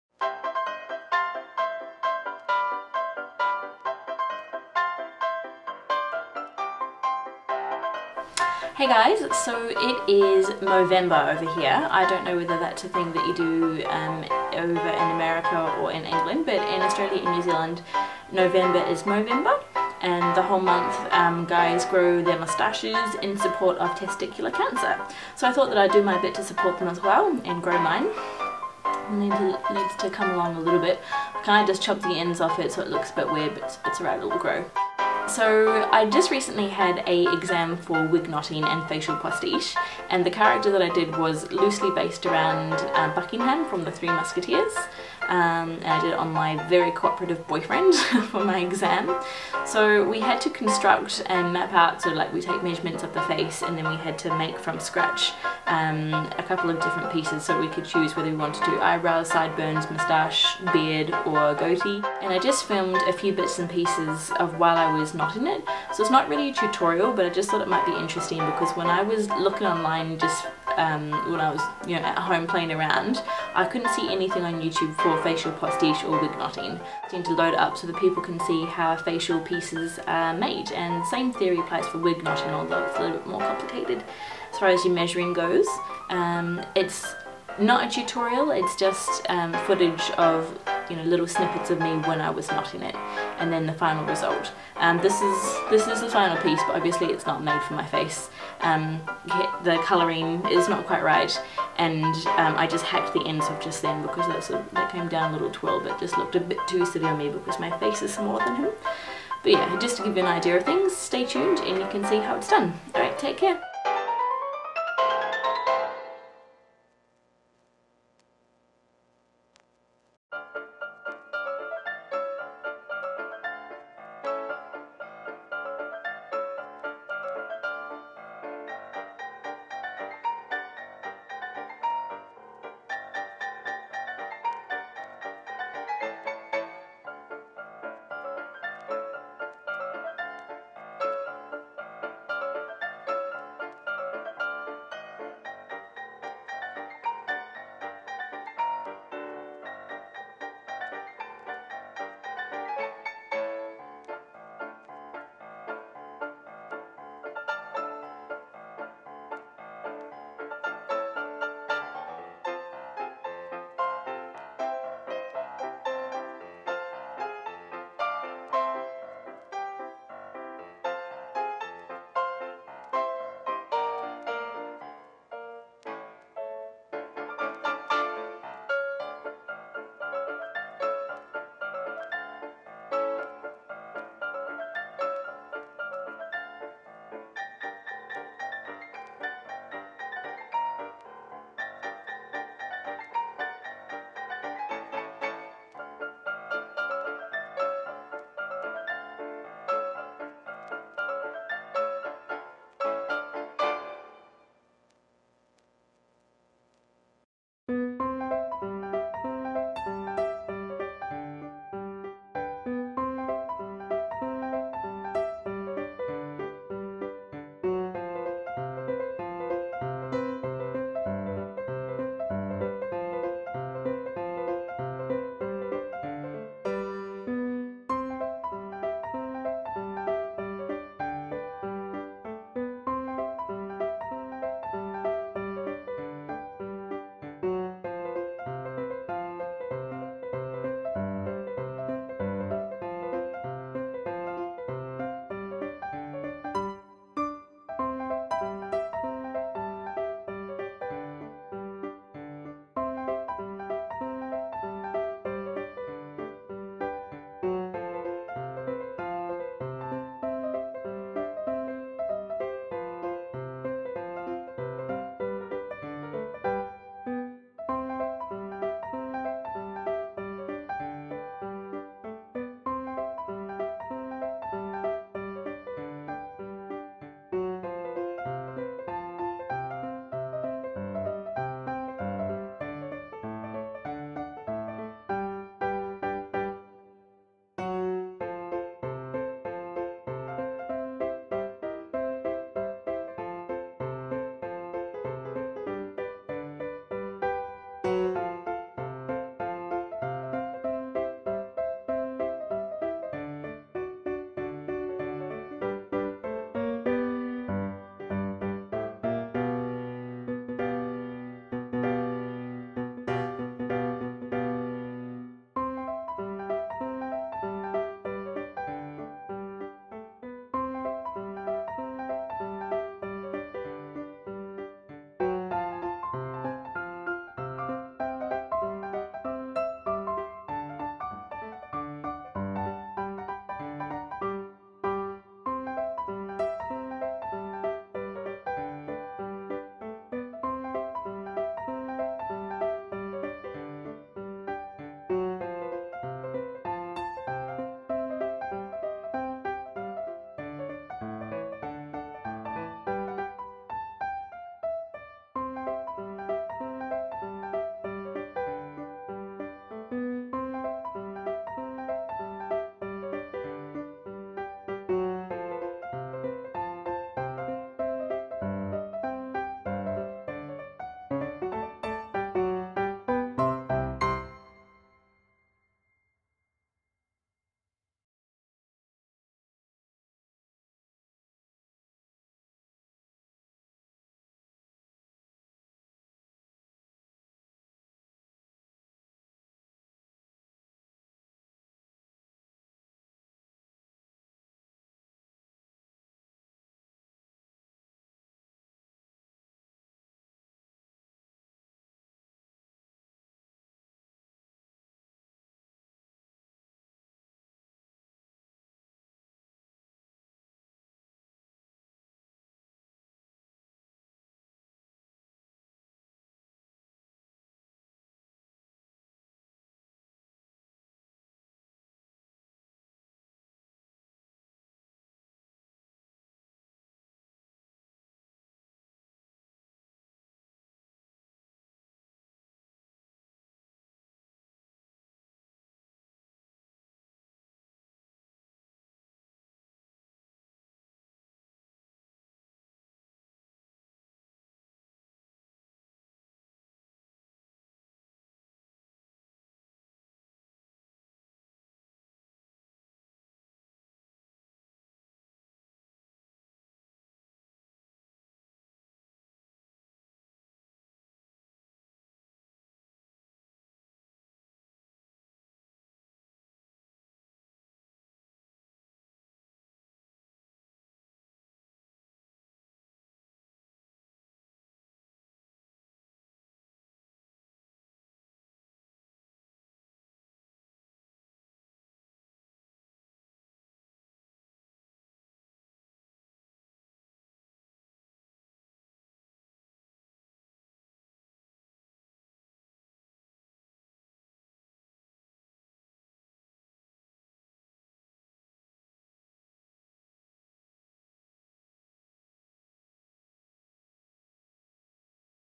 Movember - Wig knotting and Facial Postiche
I filmed a few snippets of me knotting while I was making my facial postiche for an exam, and thought since it was Mo-vember I'd load it up.

This is not a tutorial, just a quick look at how facial postiche (fake beards, eyebrows, mo's sideburns and wig's (although wig knotting is a whole other thing, and I do not know how to make one from scratch)) is made.  When I was knotting this I had a little look on youtube and couldn't see anything anywhere for it so thought I'd load it up.  If you like it and would like to know how it's done, let me know and I may make a more in-depth video about it in the future.